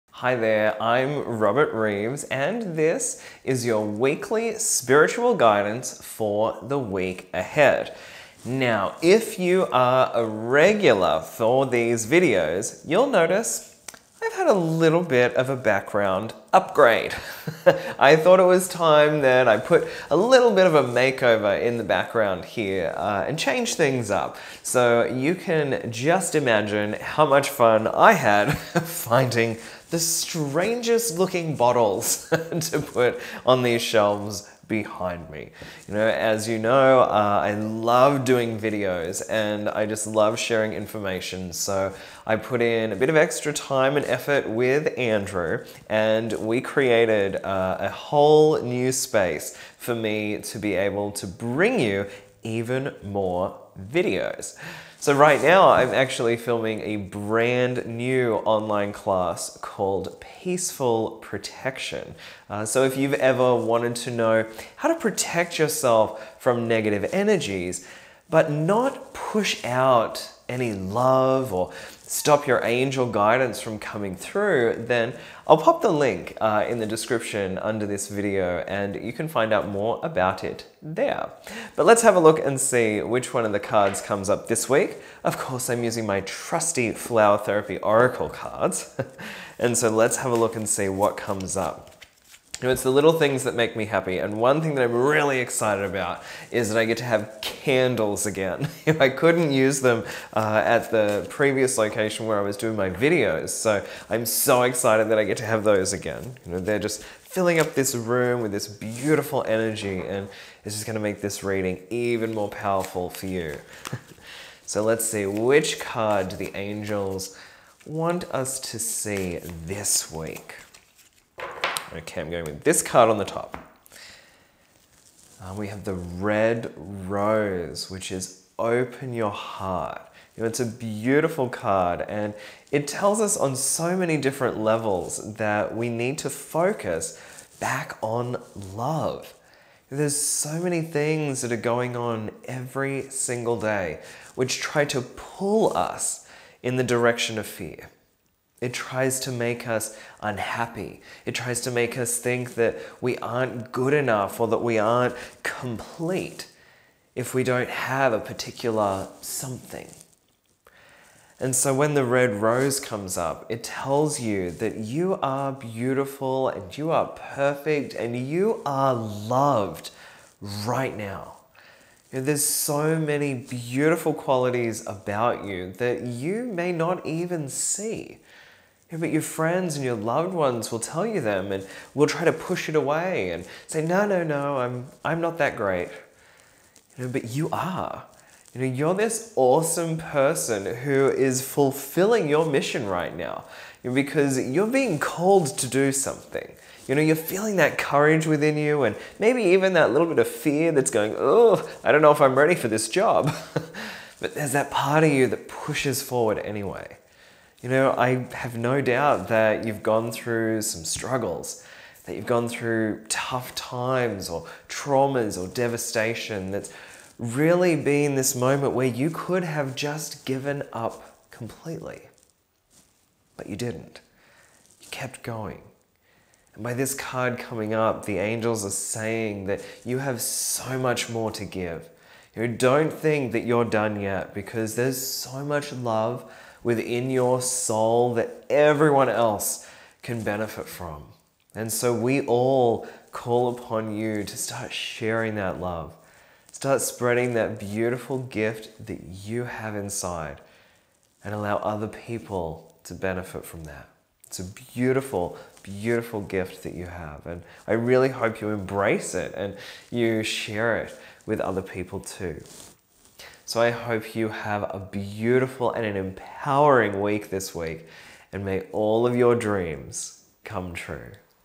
ロバート・リーブス 2016年５月30日（月）～ ６月５日（日）のウィークリーメッセージ
フラワーセラピーで知られるロバート・リーブスが毎週行っている「ウィークリーメッセージ」、2016年５月30日（月）～ ６月５日（日）分を日本語訳しました。